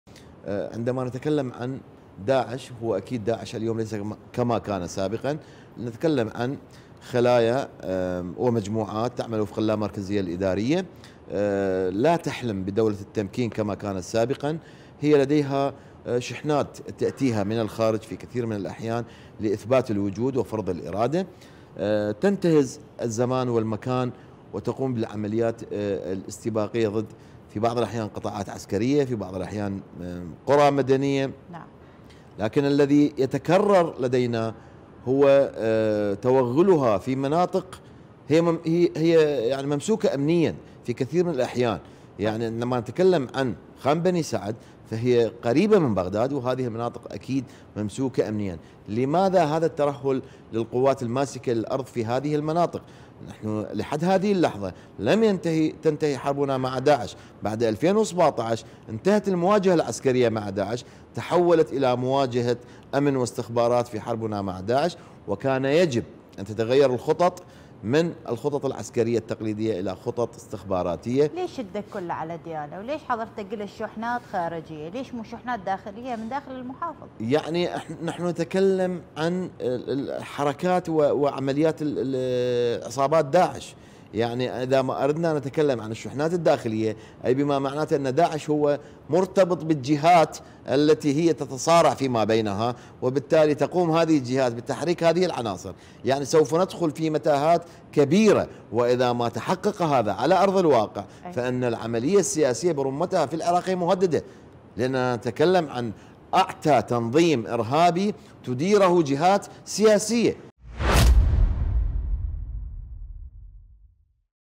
كيف توغل بديالى؟.. مخلد حازم يتهم جهات سياسية بـ"تحريك" داعش الإرهابي
مخلد حازم / خبير امني